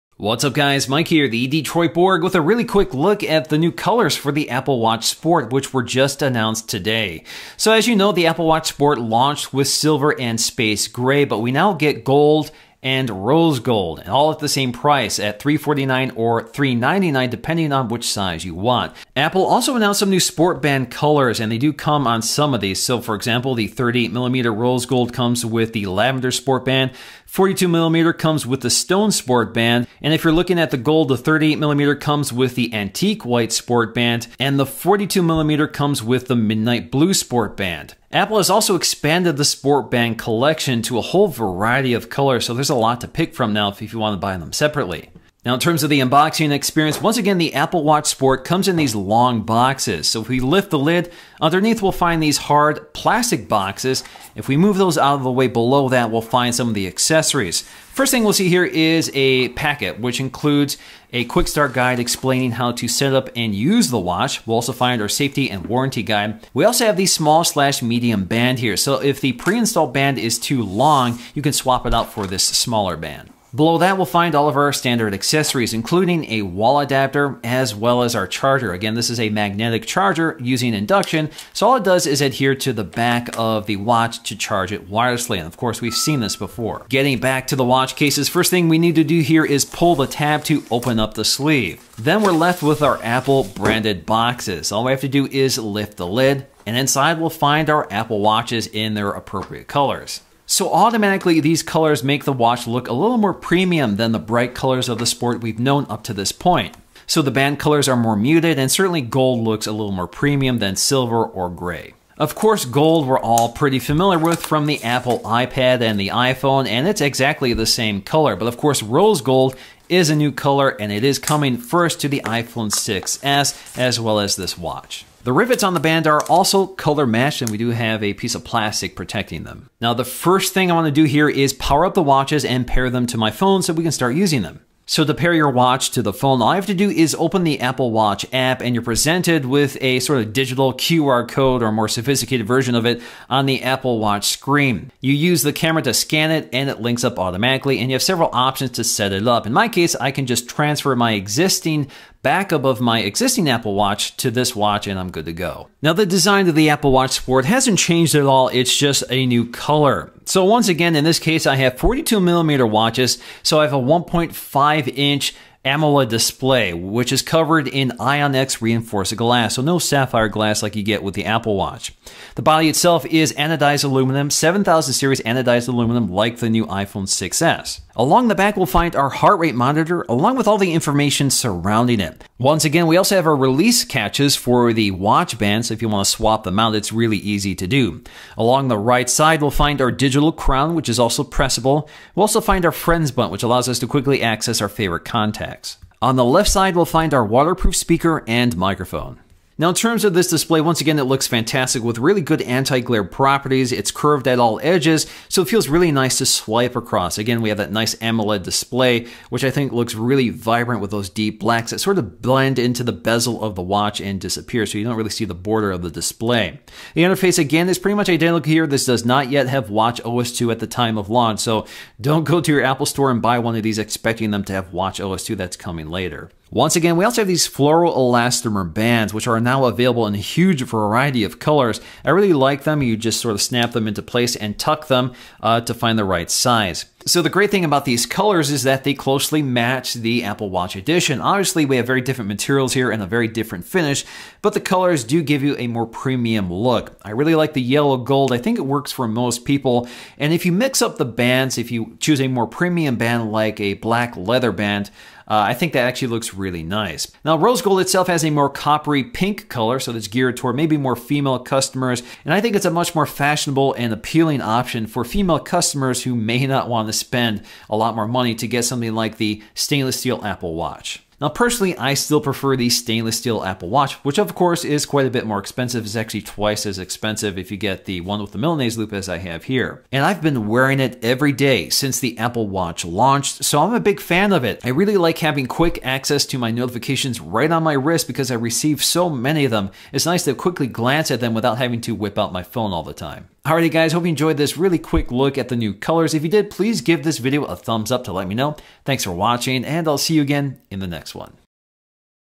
Rose Gold and Gold Apple Watch Sport (Unboxing)
Unboxing of the new Rose Gold and Gold apple watch.

Rose gold gold apple watch rose gold rose gold Tomorrowworld 2015 tomorrow world 2015 tomorrowworld